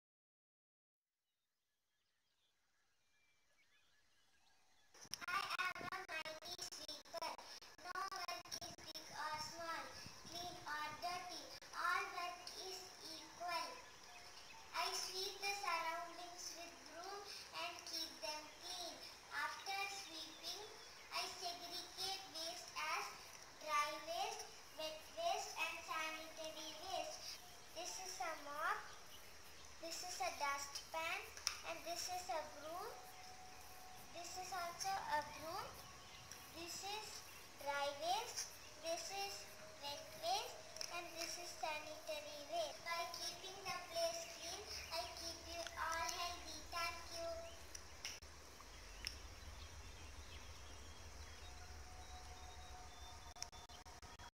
We Salute the sweeper and the Sanitary worker|A community helper|Pandemic Warriors and Our Saviors
sweepers play an important role in maintaining the health and hygiene within the cities...
 We Salute and you are our protectors always and during this COVID-19